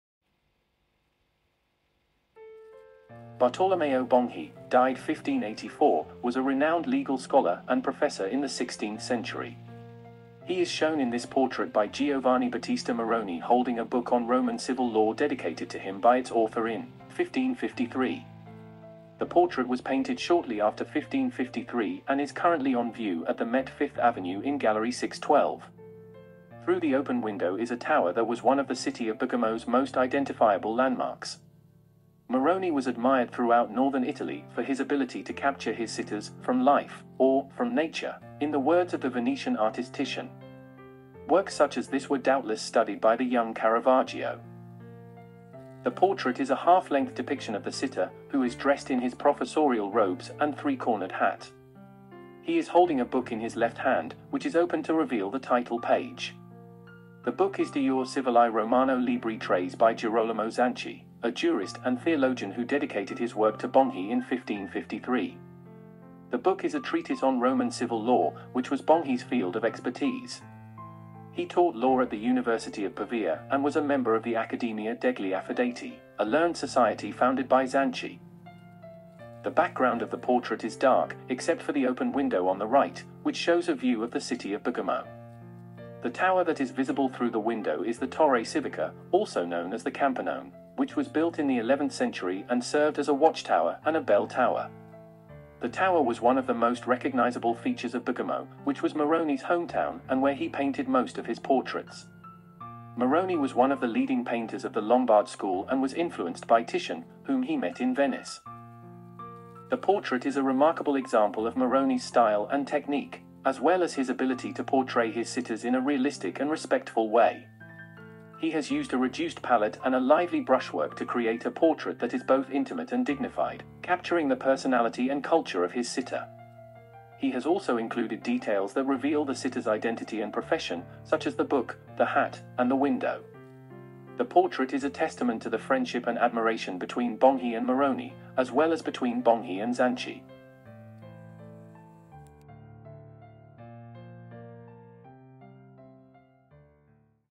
A renowned legal scholar and professor: Bartolomeo Bonghi, Giovanni Battista Moroni, after 1553
Dressed in his professorial robes and three-cornered hat, the legal scholar Bartolomeo Bonghi is shown holding a book on Roman civil law dedicated to him by its author in 1553. Through the open window is a tower that was one of the city of Bergamo’s most identifiable landmarks. Moroni was admired throughout northern Italy for his ability to capture his sitters "from life," or "from nature," in the words of the Venetian artist Titian. Works such as this were doubtless studied by the young Caravaggio.